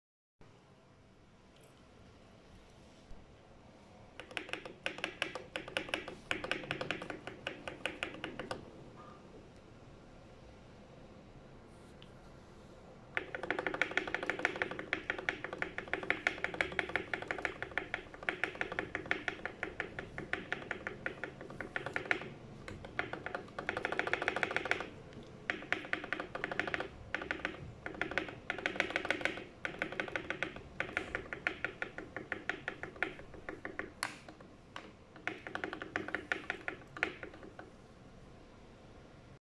creamy spacebar
mmm creamy spacebar

tm680 keyboard
foam modded
gateron phantom yellow switches
Stock stabs lubed with 205g0
random rk keycaps